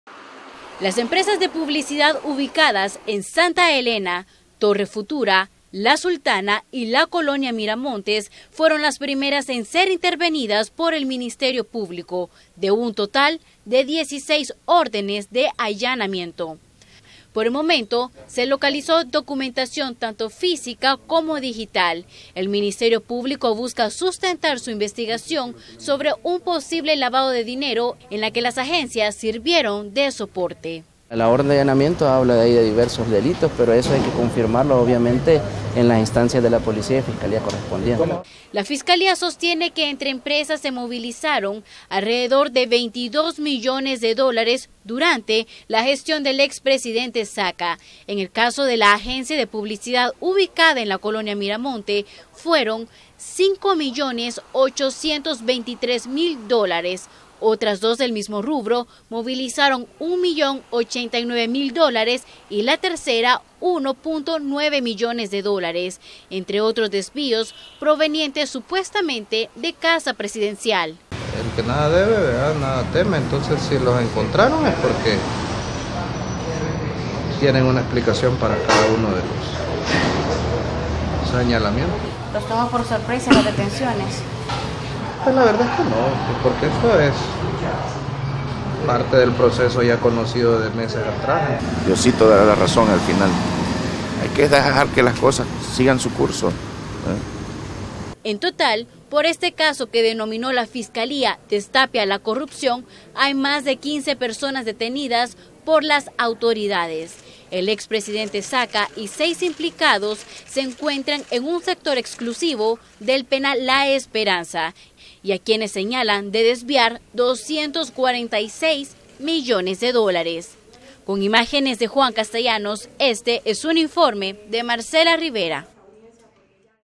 CN: Allanamientos vinculados al caso Saca
Ante estas capturas, las autoridades realizaron una serie de allanamientos en empresas de publicidad. Veamos.